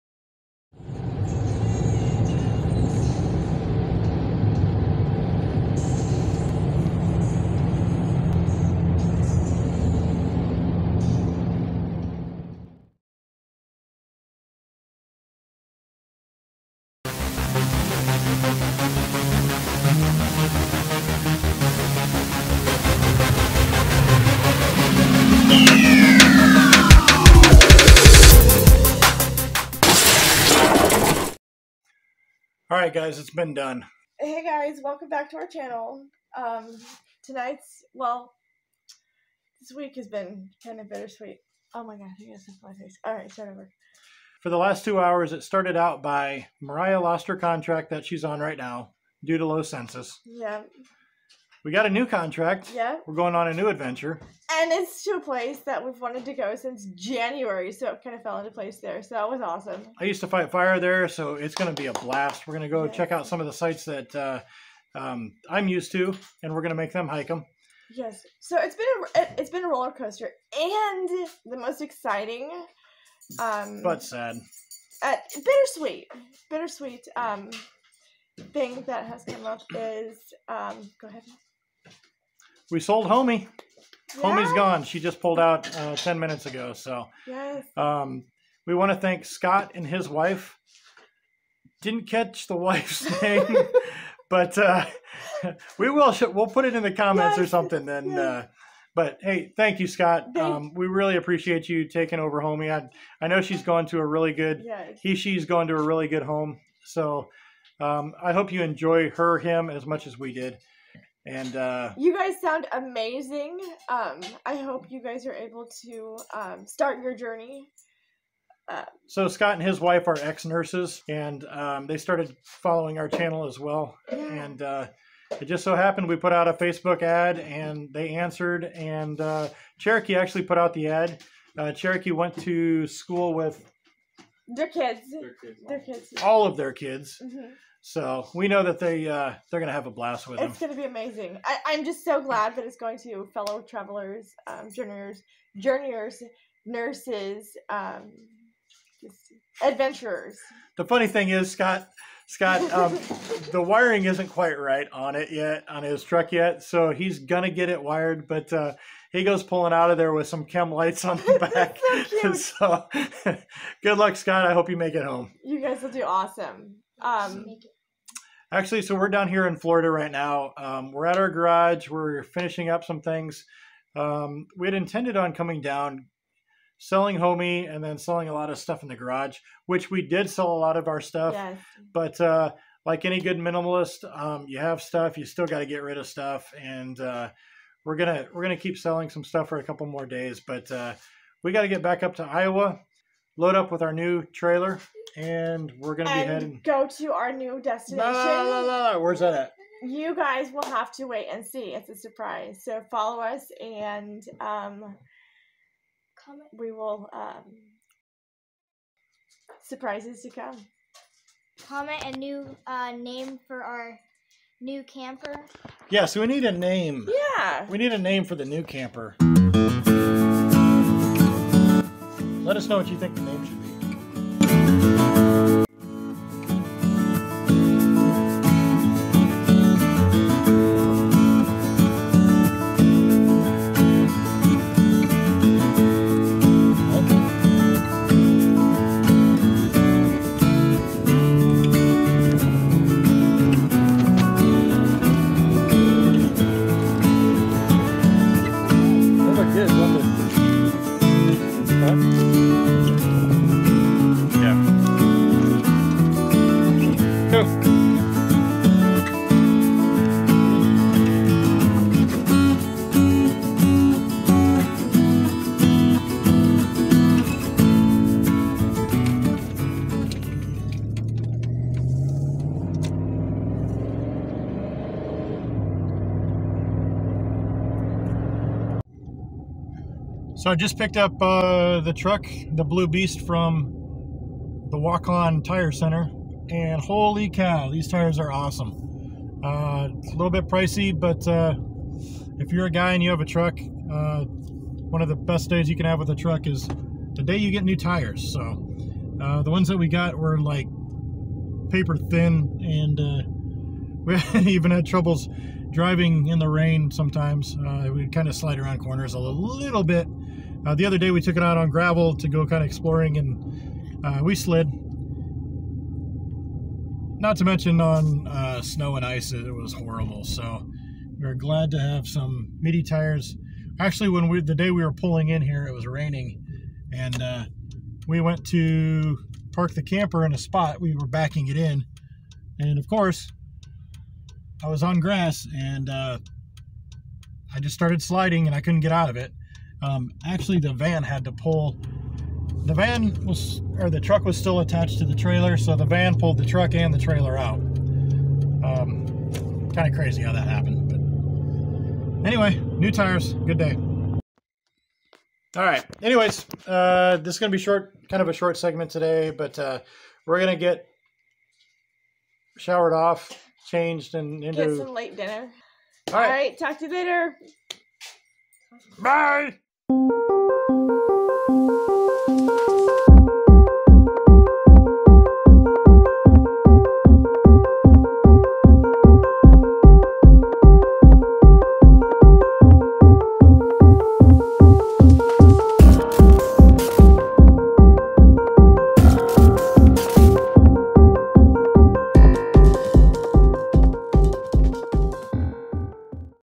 Homie is Gone... and the New Tires are ON!
Today is our quickest episode yet.  Sorry but we just traveled across the country to sell our first RV, "Homie", and our heart goes along with it.  It's going to take some time to get over this, but we have to focusing on our new home.   We also spend 20 hours working on our storage garage that we rent in Florida and list, sell, donate and load items that we will be bringing with us on the road.  Jeff gets a new set of tires for the "Blue Beast" and takes them out for a spin.